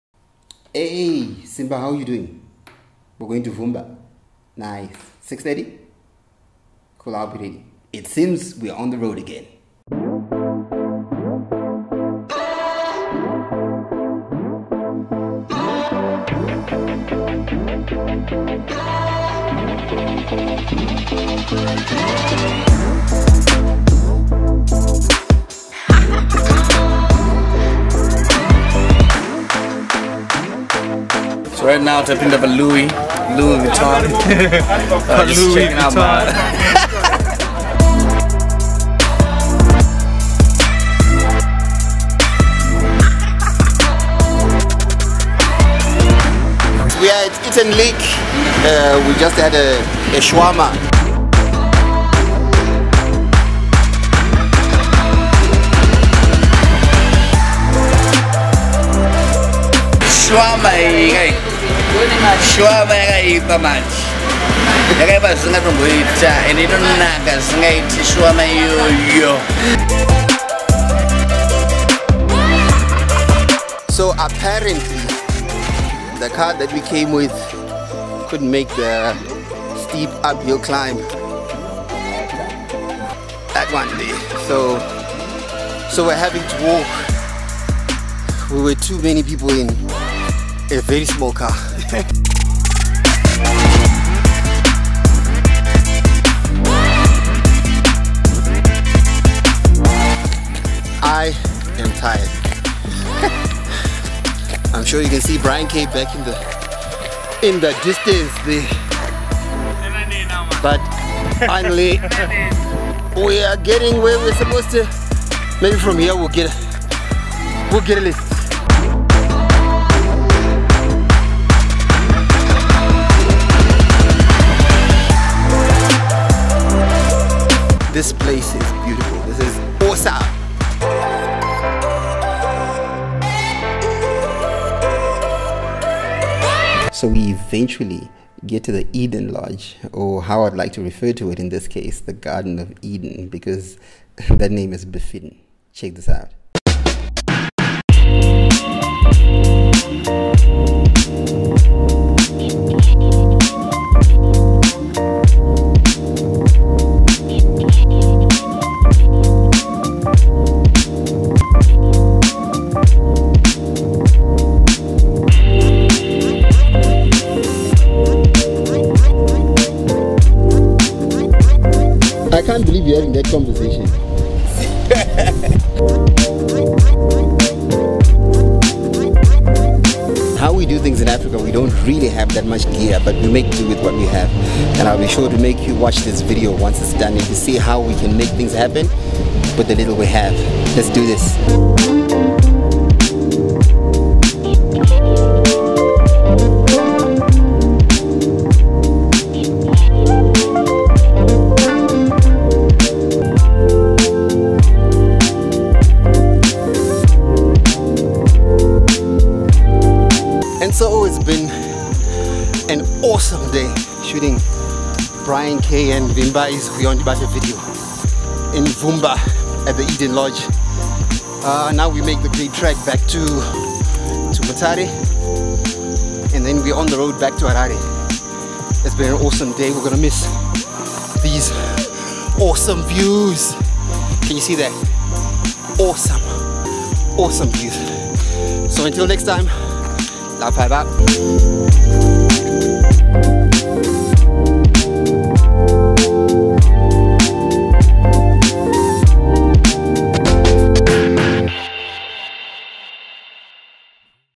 How we got to the Garden of Eden
These are VLOG experiences of behind the scenes shoot for Vimbai ZImuto and Brian K's music video directed by Simba Gee,